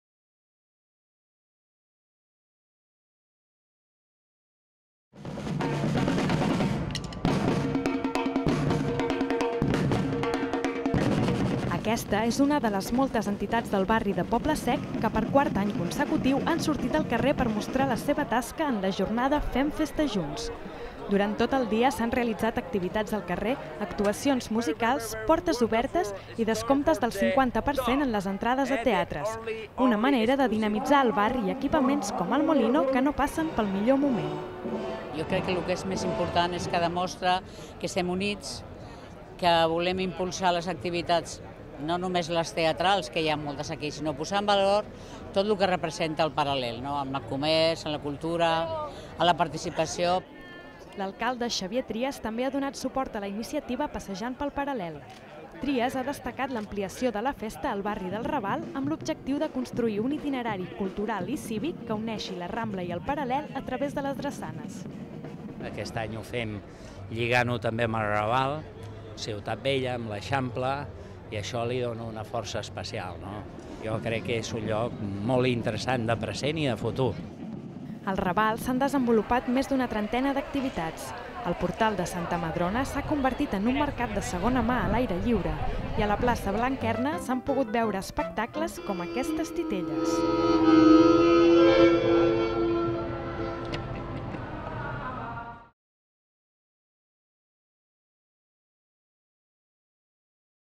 Fem Festa Junts uniex el Paral·lel i el Raval
Per quart any consecutiu entotats, associacions i veïns del Poble Sec han sortit al carrer al Fem Festa Junts. Enguany s'hi ha sumat també el Raval Sud. Al Portal de Santa aMadrona s'hi ha instal·lat un mercat de segona mà a l'aire liure i dos escenaris on s'hi ha pogut veure des de danses de Bollywood a titelles.